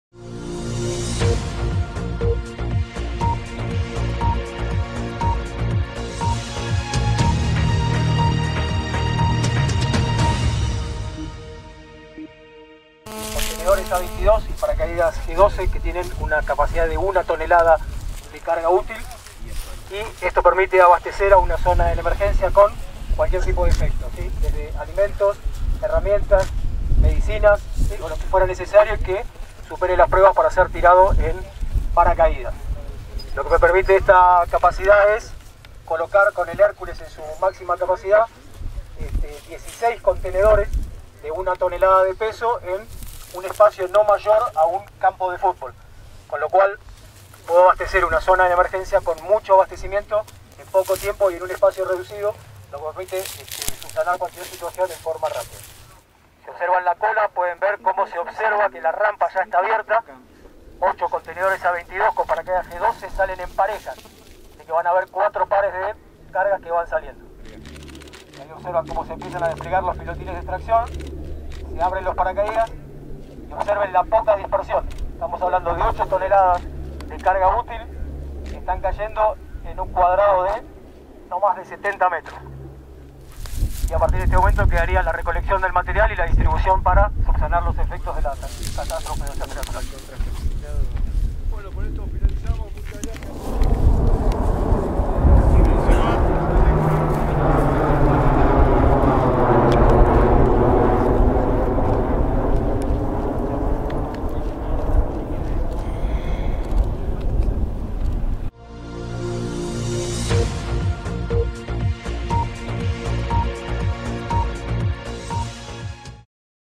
Ejercicio Solidaridad 2016: aero-cooperación
SMAP - 2016
Este ejercicio tuvo lugar en cercanías del Aeropuerto Carlos Campos, donde desde un Hércules C-130, se hizo el lanzamiento de 8 cargas de una tonelada cada una, que permite en zonas aisladas, abastecerlas con alimentos, medicinas o herramientas, en una zona reducida, no mayor a un campo de futbol. Los 8 contenedores A-22 con sus paracaídas, son lanzados desde la aeronave, cayendo con una baja dispersión, que luego es recolectada y distribuida por personal en tierra.-